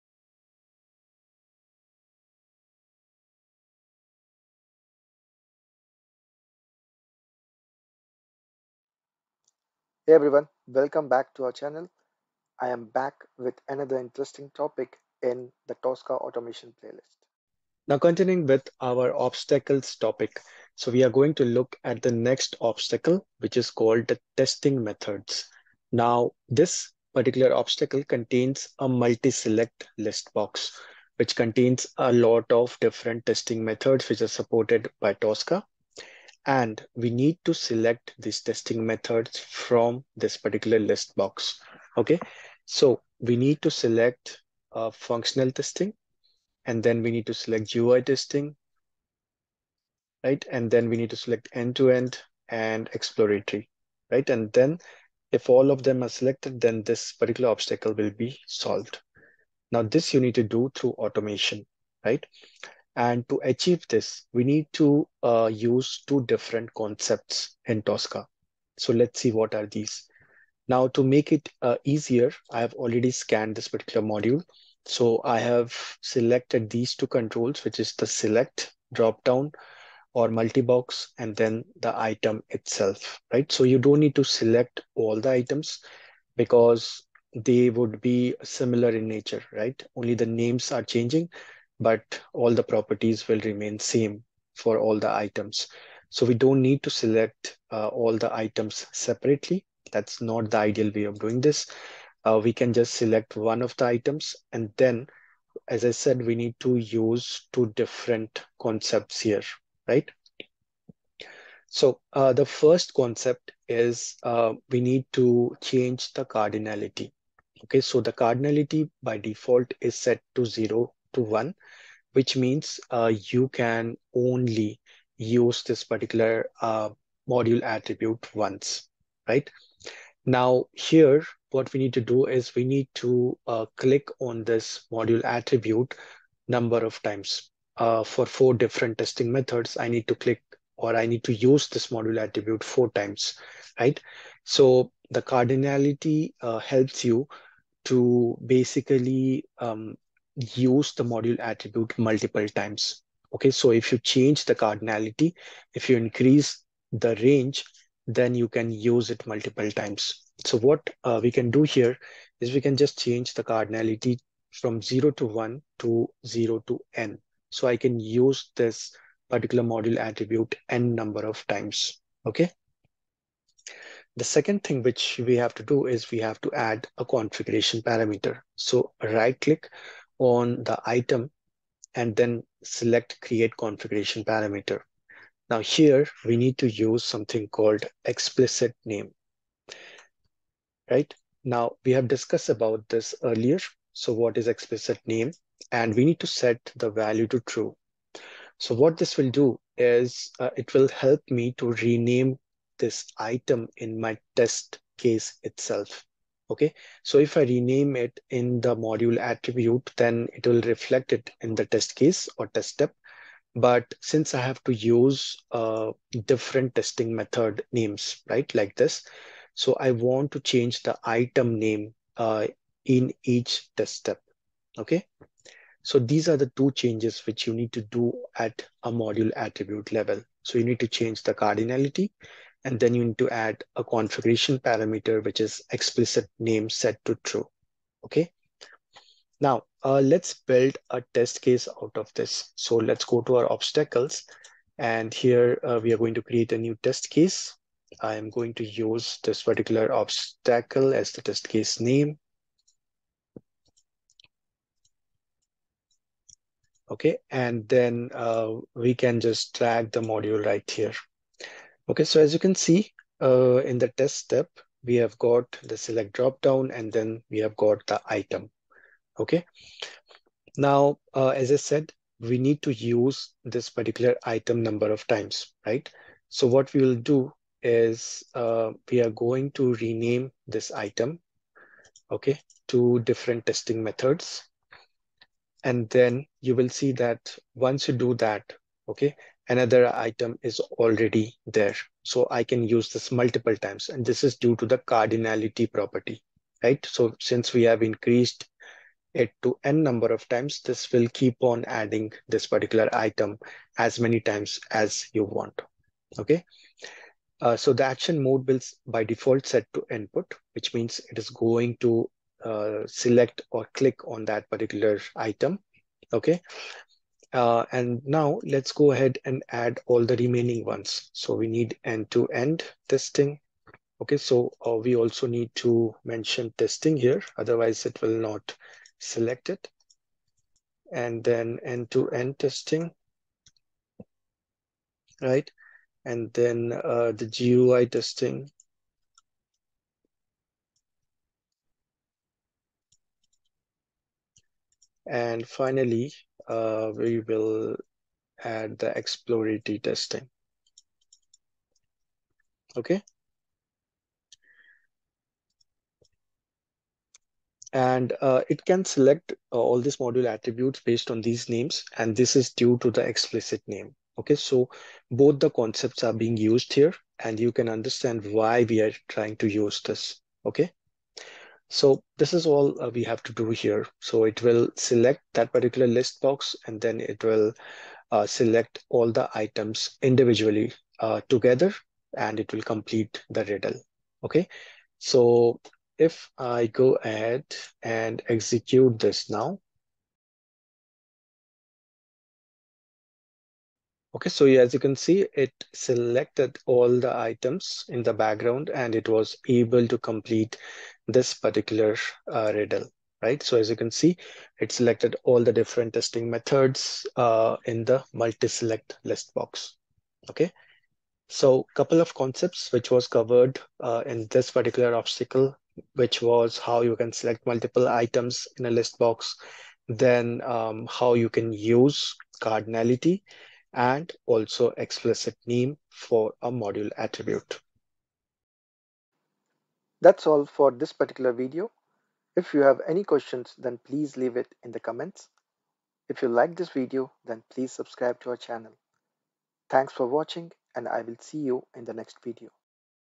Tosca Tutorial | Lesson 112 - Multiselect ListBox | Cardinality | Explicit Name | Obstacle 6
Step-by-step guide on how to select multiple items in a Multiselect listbox using cardinality and Explicit Name Configuration Parameter

Obstacle Details:
Name - Testing Methods
Description -  Multiselect listbox: Select all the testing methods, that are supported by Tosca (Functional, End2End, GUI testing and Exploratory Testing)
Challenge - Need to use module attributes multiple times and rename
Solution: Use cardinality and explicit name configuration parameter